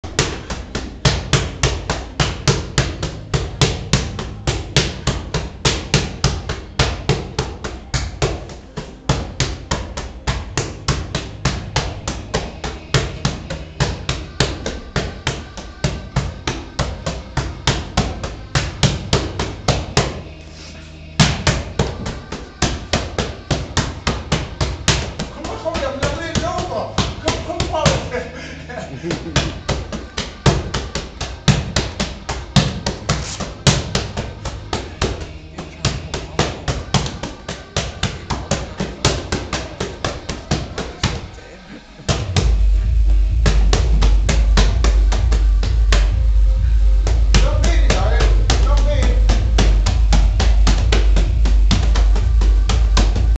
Boxing 30th March 2013
Boxing yesterday with 3X World Champion Carl 'The C.A.T' Thompson.